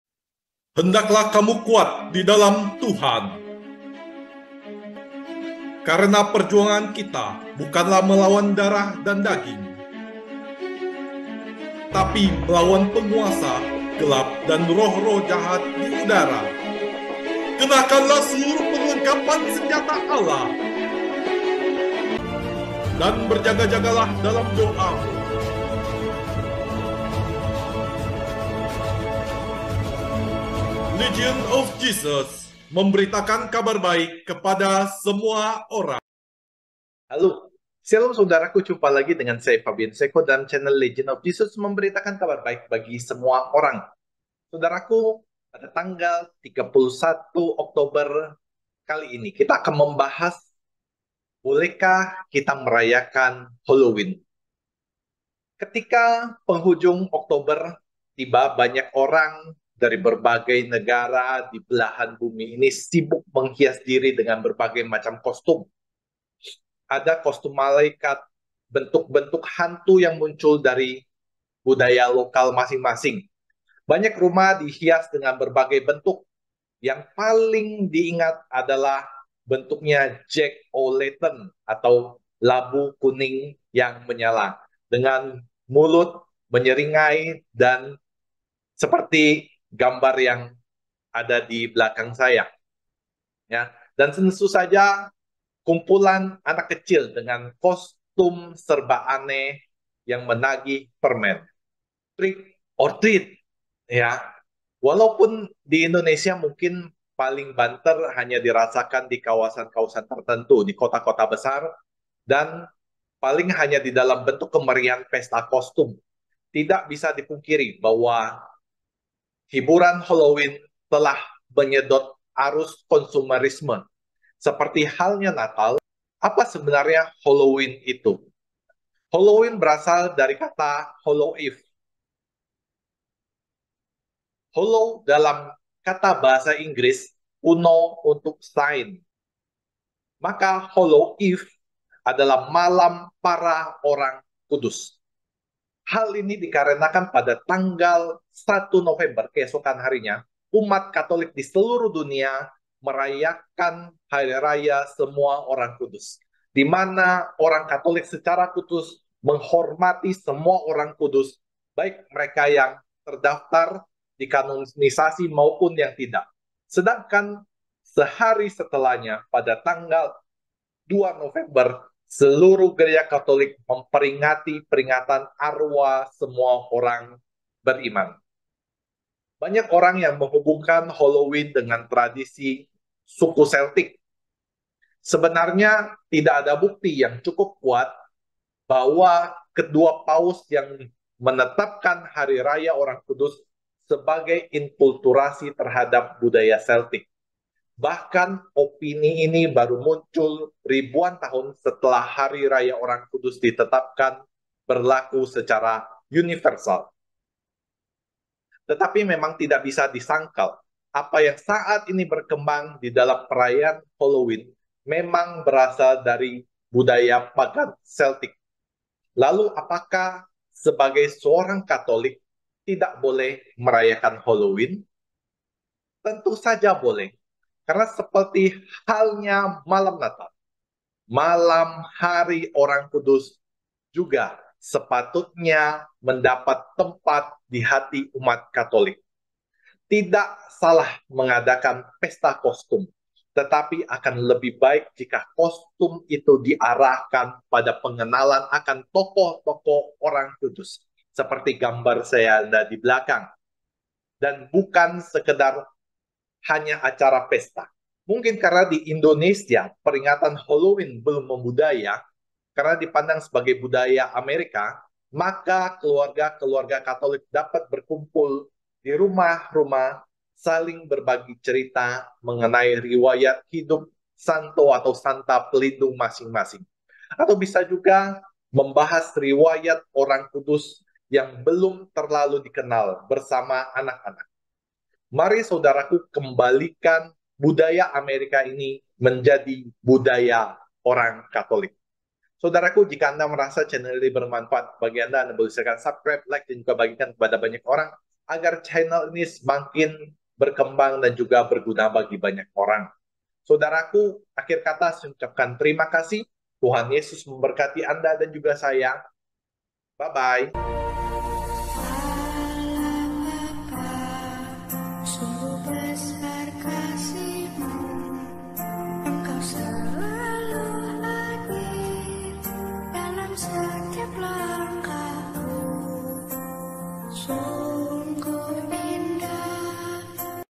✝️HALLOWEEN BOLEHKAH DIRAYAKAN❓❓❓‼️‼️ (#197)
Shallom,
Memasuki Akhir bulan Oktober biasanya akan dirayakan hari raya Halloween terutama dalam budaya barat, apakah perayaan Halloween boleh dirayakan oleh umat Katolik???

MARI KITA SIMAK SAMPAI HABIS AGAR TIDAK TERJADI KESALAHAN DALAM PEMAHAMAN...

LEGION OF JESUS
MEMBERITAKAN KABAR BAIK BAGI SEMUA ORANG